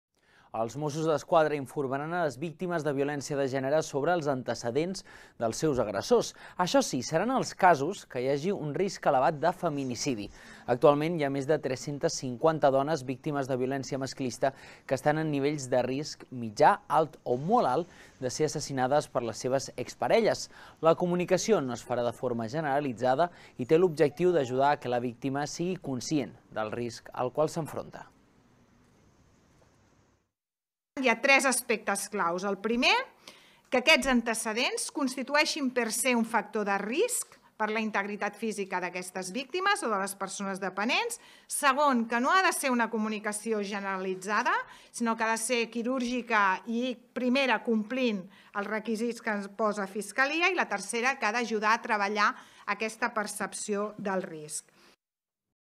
Els Mossos informaran les víctimes de violència de gènere sobre els antecedents del seu agressor
Els Mossos d'Esquadra informaran les víctimes de violència de gènere sobre els antecedents del seu agressor. Això sí, serà en els casos que hi hagi un risc elevat de feminicidi. Actualment, hi ha més de 350 dones víctimes de violència masclista que estan en nivells de risc mitjà, alt o molt alt de ser assassinades per les seves exparelles. La comunicació no es farà de forma generalitzada i té l'objectiu d'ajudar al fet que la víctima sigui conscient del risc al qual s'enfronta.